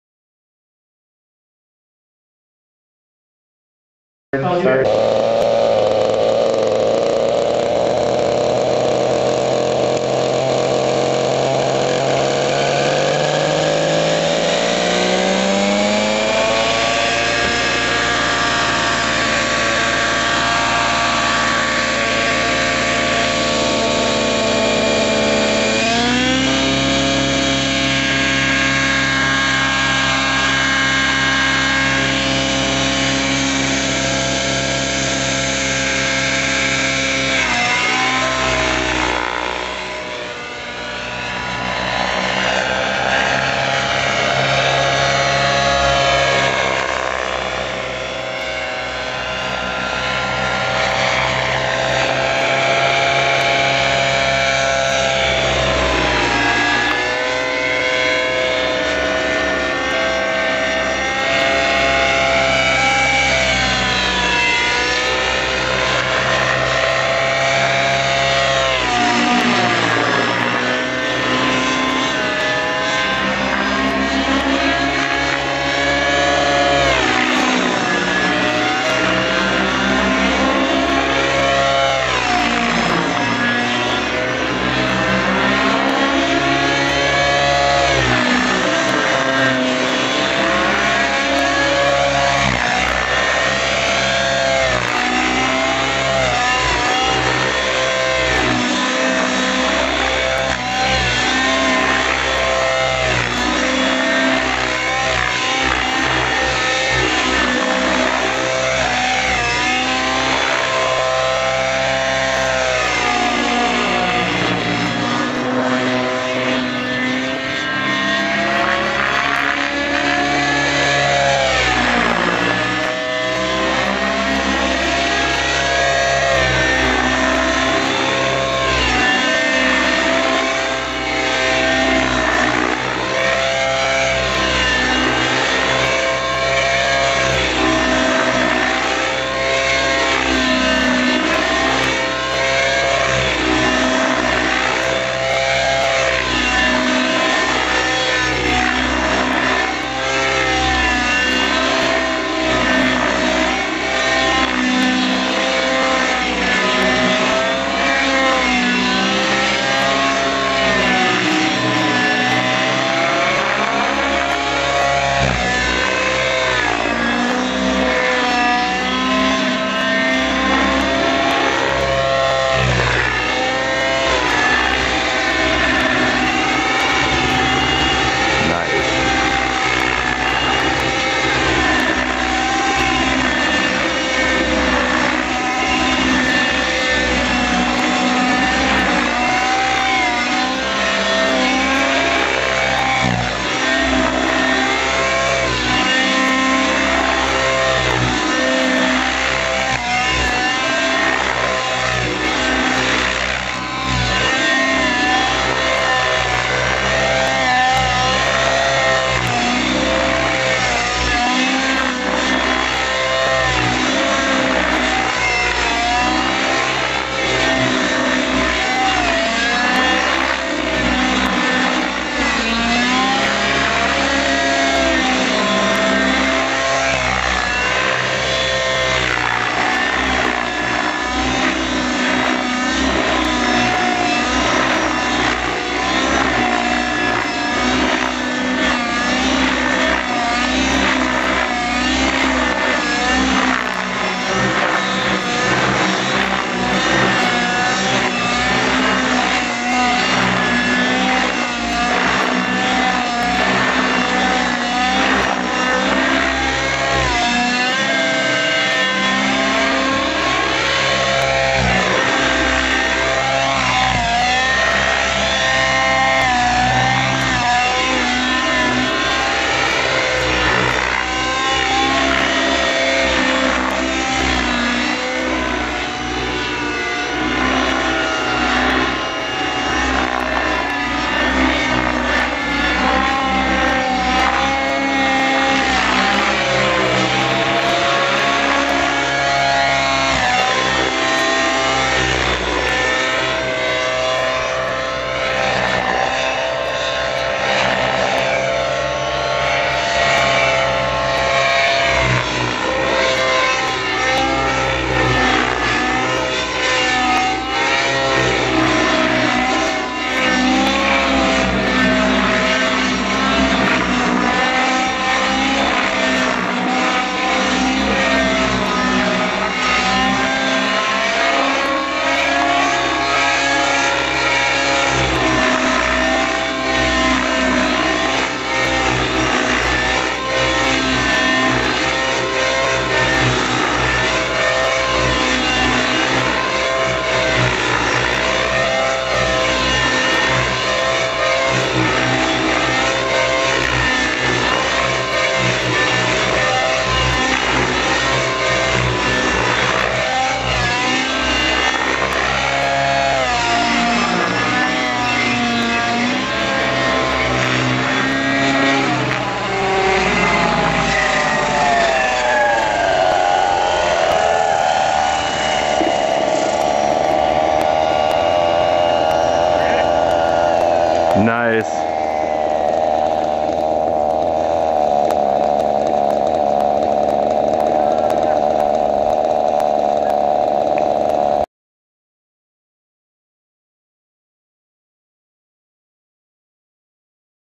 Eric Brandenburg Stratus flying
Some smooth and precise 3d on my old high ratio Stratus.